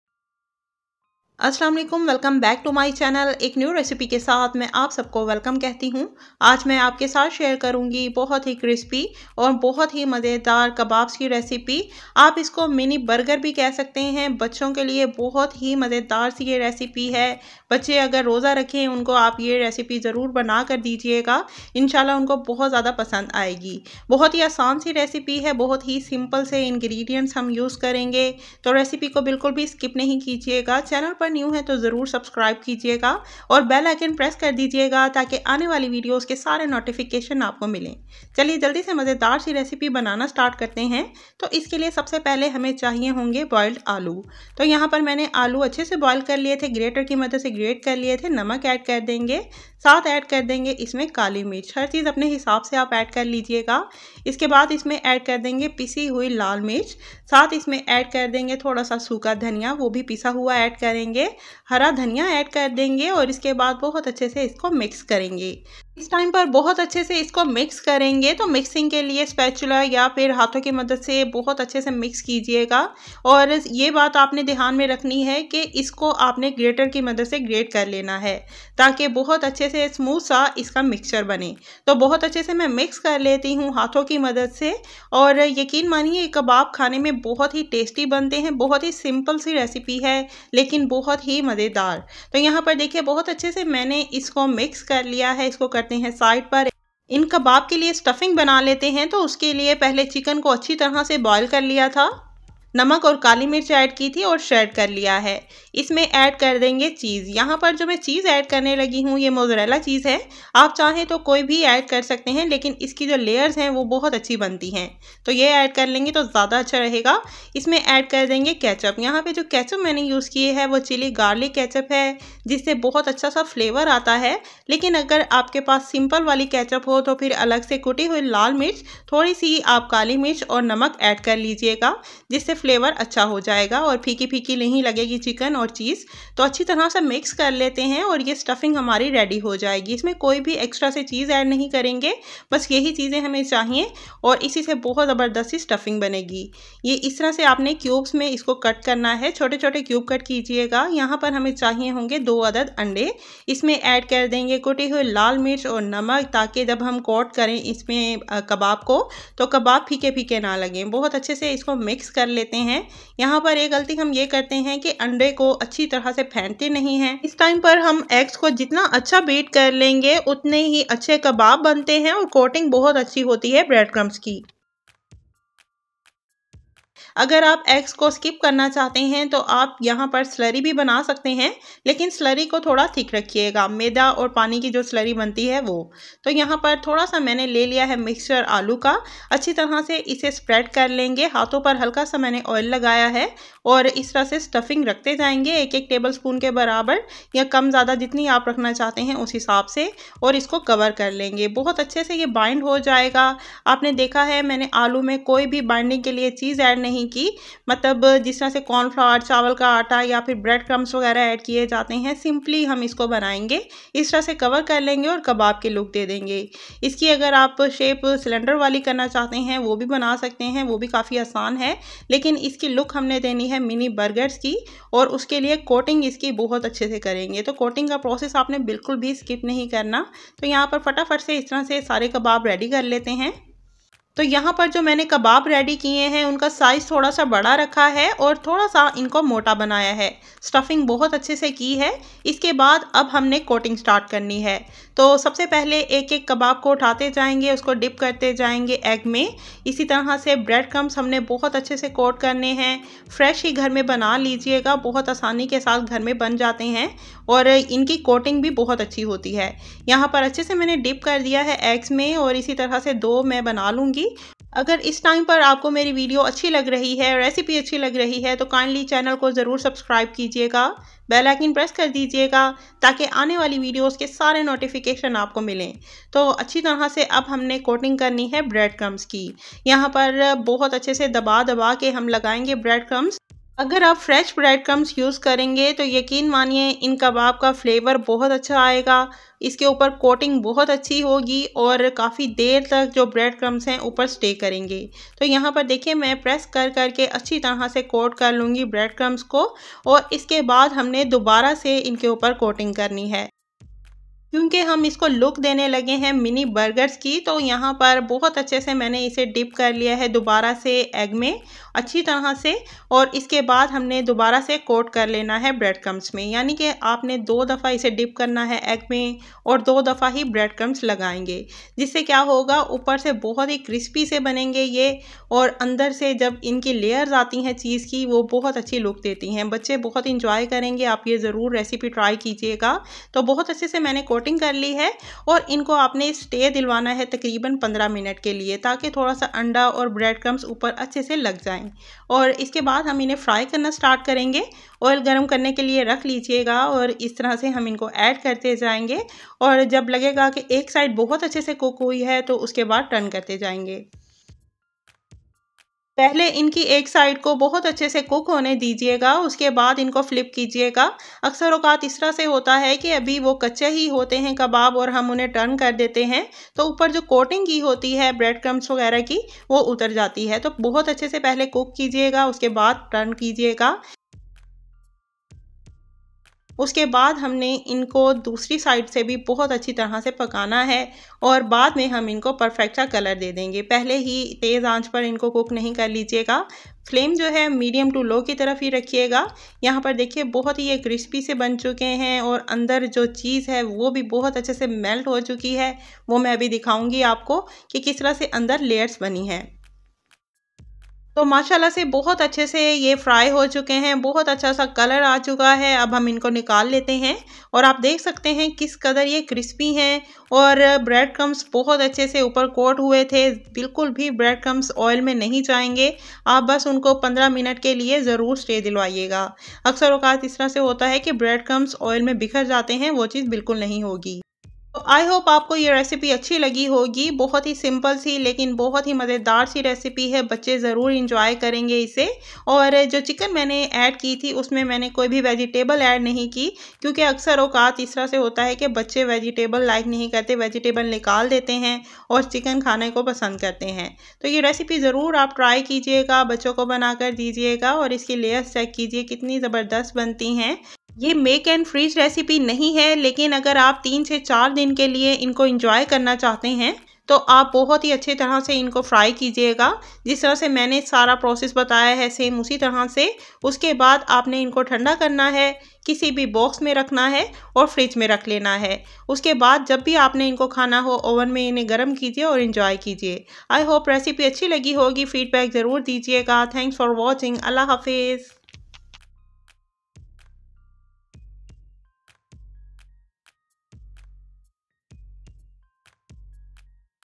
Crispy Potato Cheese kabab recipe | Potato Cutlets 2024 | Stuffed cheese kabab | Multani Tarkaa
In this video I will share Crispy Potato Cheese kabab recipe | Potato Cutlets 2024 | Stuffed cheese kabab | Multani Tarkaa

➡️perfect Zarda recipe